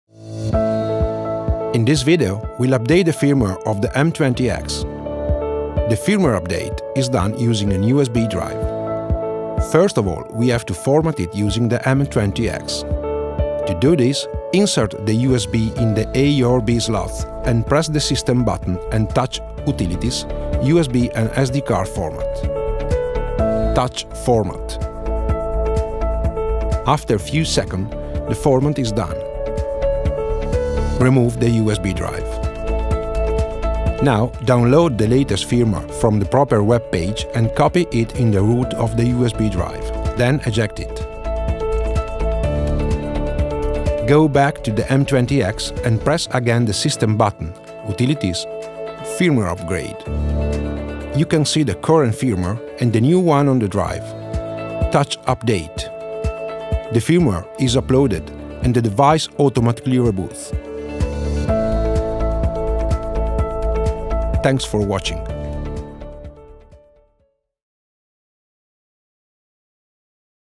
RCF M 20X DESKTOP DIGITAL MIXER - FIRMWARE UPDATE
M 20X is a fully recallable mixer with 16 remote-controlled preamps and 11 motorized faders. Despite its dimensions, the M 20X packs a whole suite of capabilities, such as high-profile preamplifiers, top-of-class DSP with studio-quality processing, WiFi remote control, and a comprehensive set of multitrack recording and playback functions. The mixer offers 20 analog inputs and 14 output channels between the balanced analog outputs and the AES/EBU digital output.